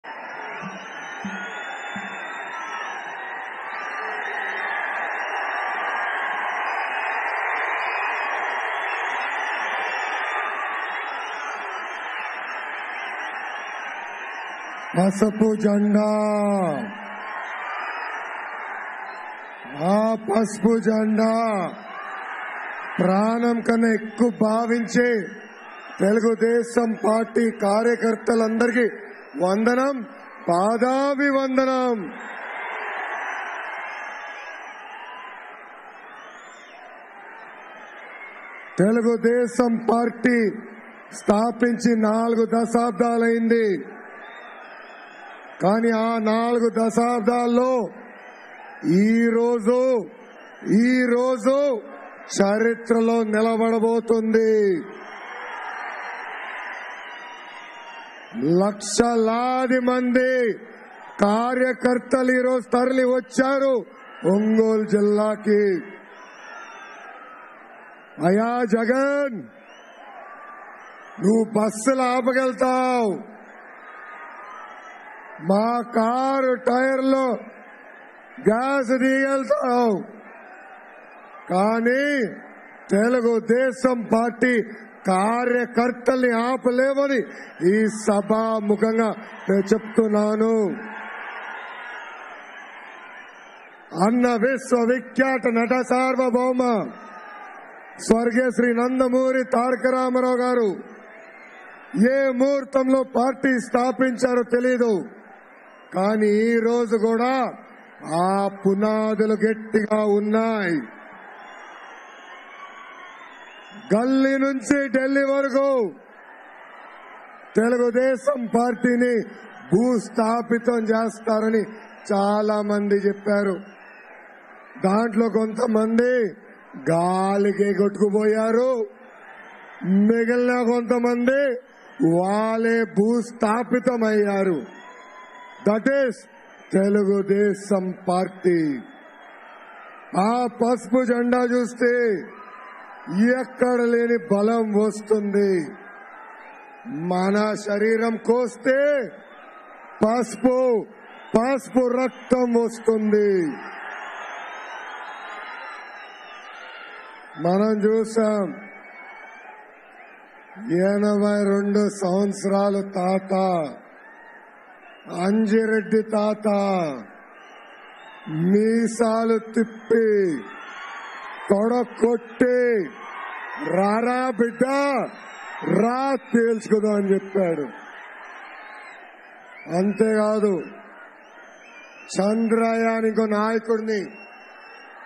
మీసం తిప్పి తొడకొట్టిన నారా లోకేష్ | Nara Lokesh Speech In Mahanadu 2022 | Ongole | TV5 News
LIVE: TDP Mahanadu 2022 (మహానాడు) || Chandrababu || Nara Lokesh || Day -2 ||  TV5 News